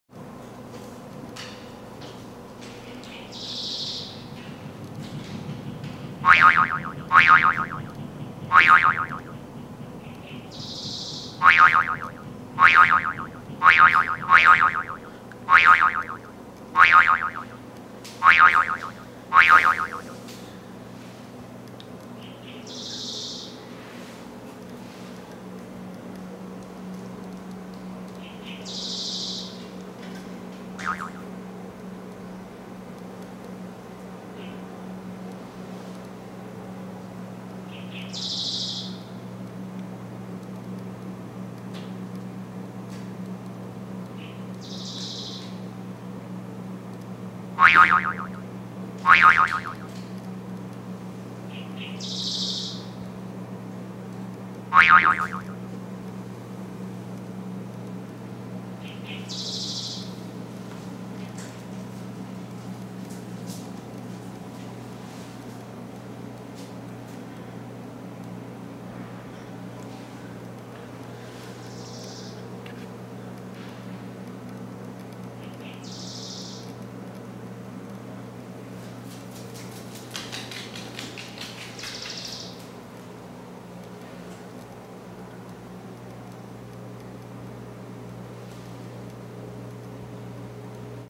My New Friend
This cute guy has been visiting me several times a day since we moved here.
Looks like he wants me to invite him in! I just had to add the boings ;-)
I've looked in all my guides to see what kind of birdie he is but I haven't had any luck identifying him. Will you help me?